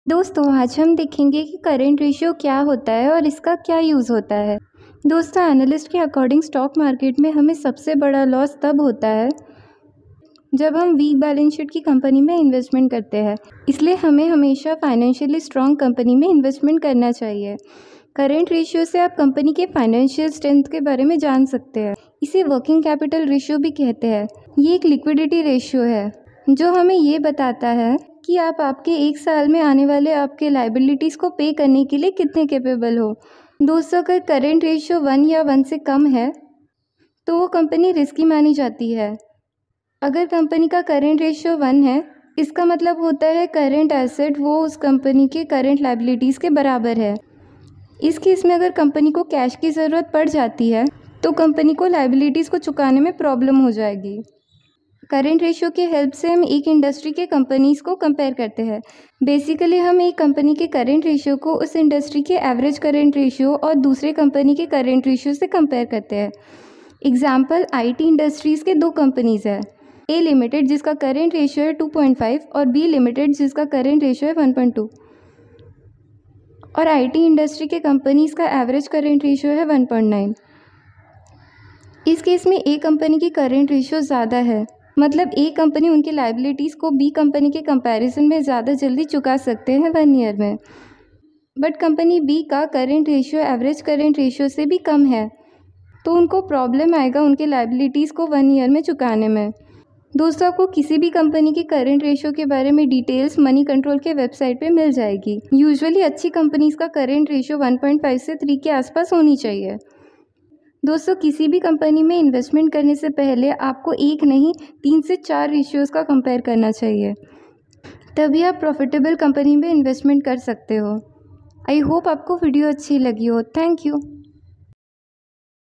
myequityschool What is Current ratio ?
in this video we have explained current ratio and it's uses